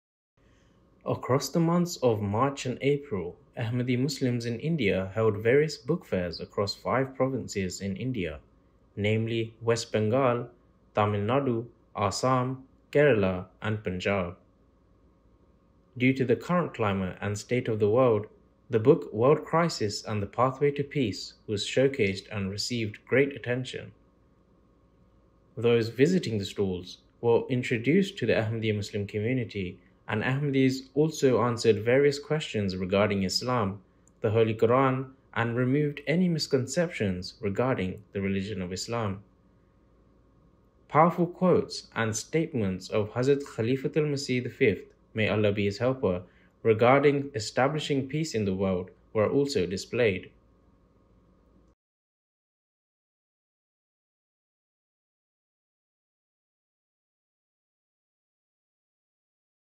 Ahmadi Muslims in India Set up Book Stalls Across The Country | MTA News
Ahmadiyya Muslim Jama'at.